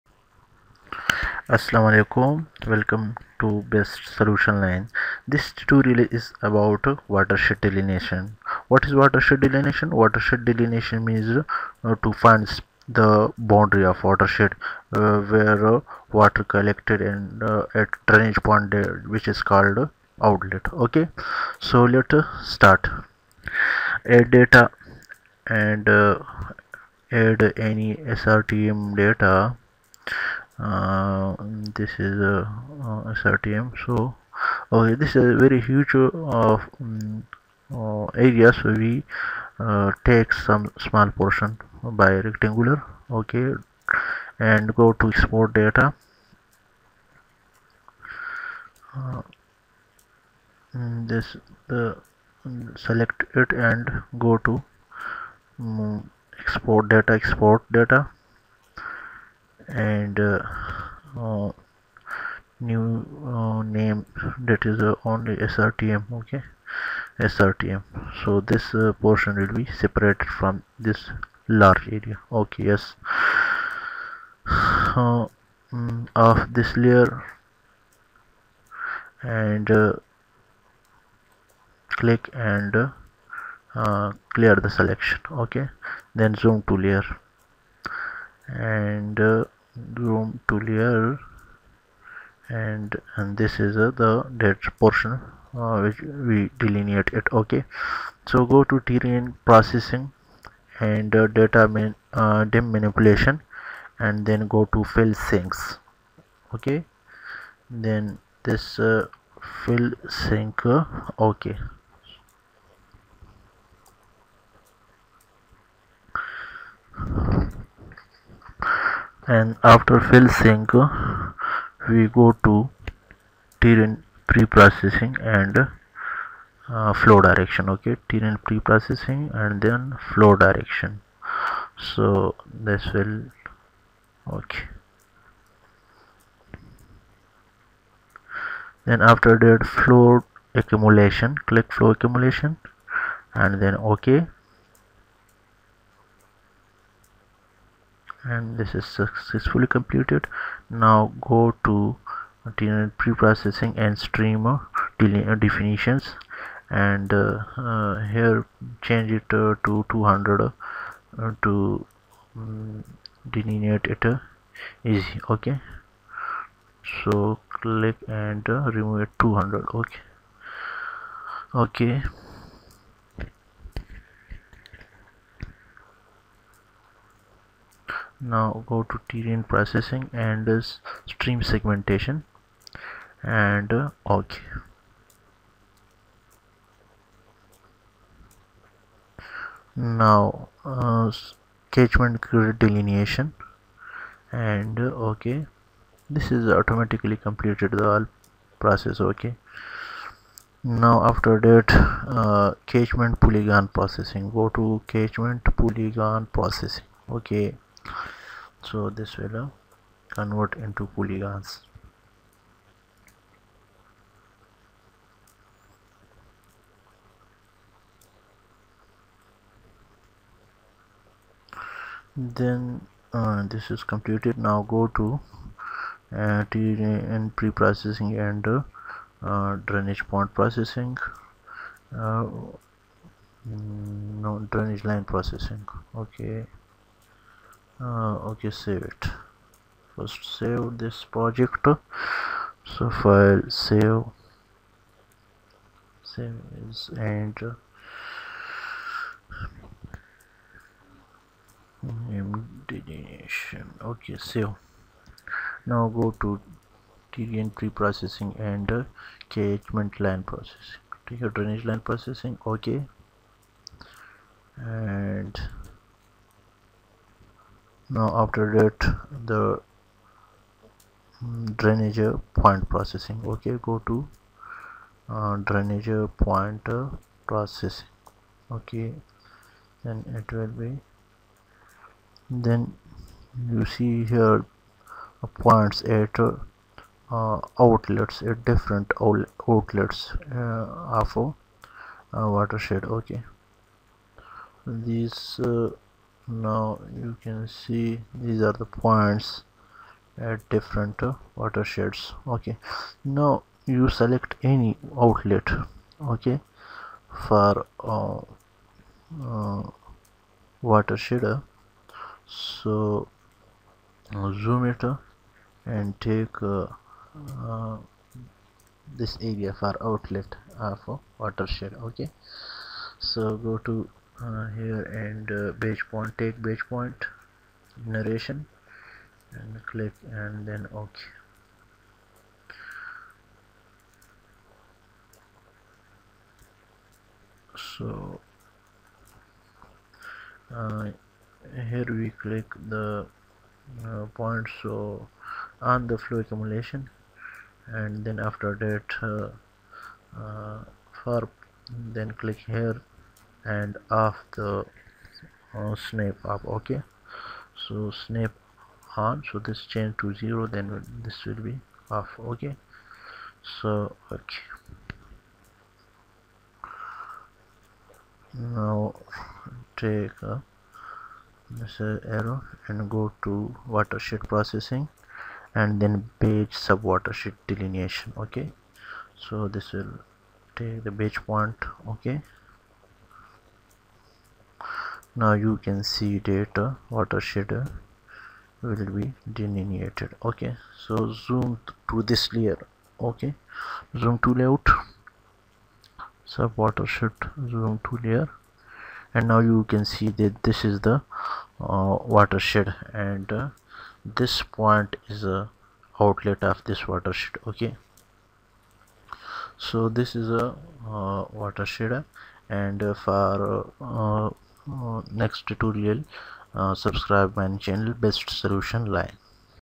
Watershed Delineation in GIS Very Easy Method
In this tutorial you will learn about that how Watershed Delineate in ArcGIS.
Watershed, the region drained into a river, river system or to any other downstream region."
Watershed delineation means is that you're drawing lines on a map to identify a watershed's boundaries. These are typically drawn on topographic maps or DEM using information from contour lines.
Watersheds are always physically delineated by the area upstream from a given outlet point. This generally means that for a stream network, the contributing area upstream to a ridge line. Ridge lines separate watersheds from each other.
This is very easy method hope you will find it best for learning.